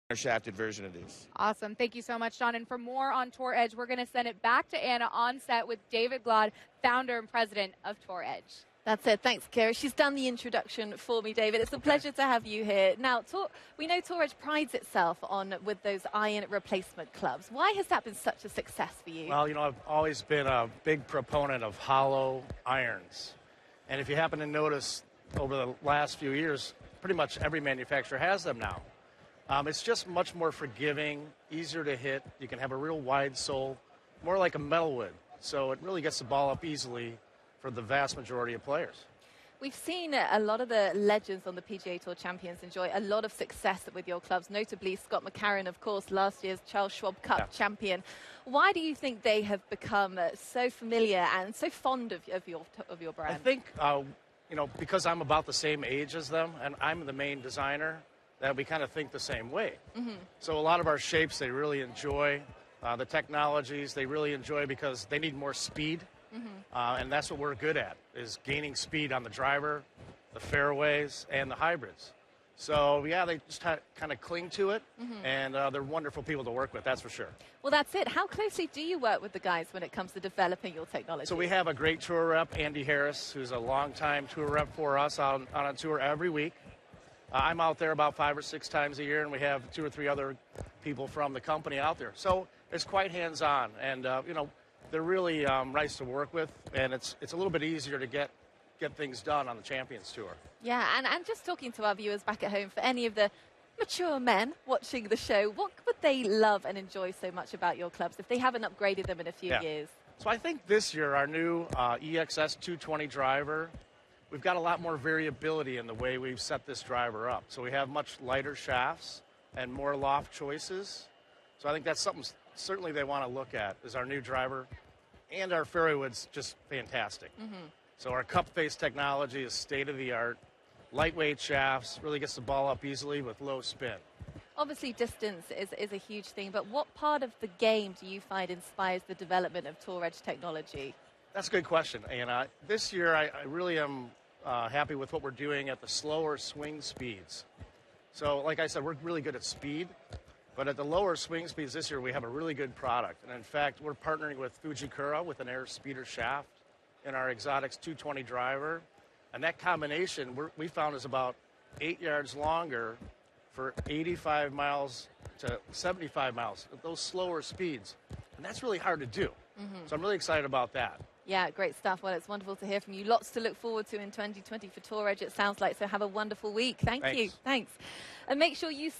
Tour Edge President and Founder David Glod Golf Channel interview from the 2020 PGA Merchandise Show
President of Tour Edge, David Glod, took to the Golf Channel stage at the 2020 PGA Merchandise Show to speak about our new product lineup, Tour Staff players and the many exciting happenings at Tour Edge!